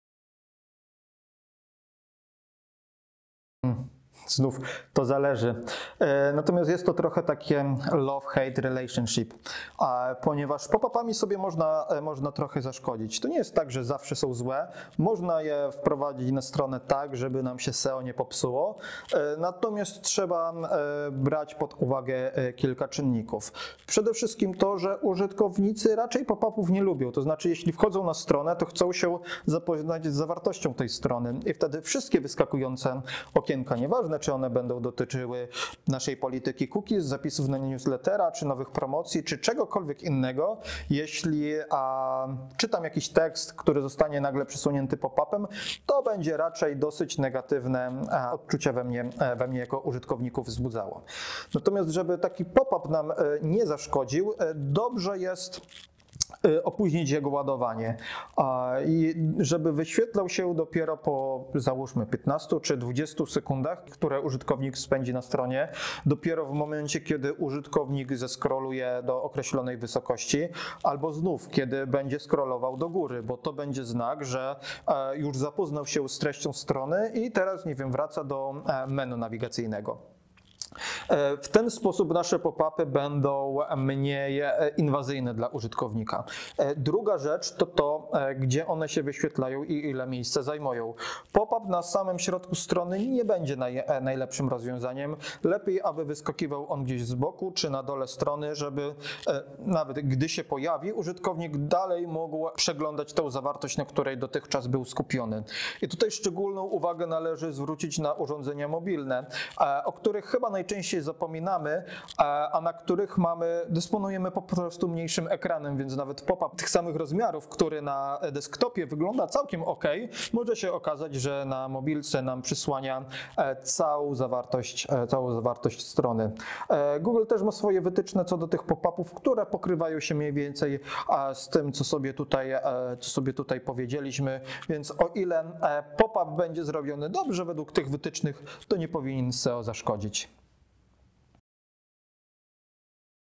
Jak Pop-upy wpływają na SEO? | 60 dni z marketingiem #31 Wojciech Urban
Jak Pop-upy wpływają na SEO? 31. odcinek wyzwania 60 dni z marketingiem

Masz jakieś pytania? Zadaj je w komentarzu!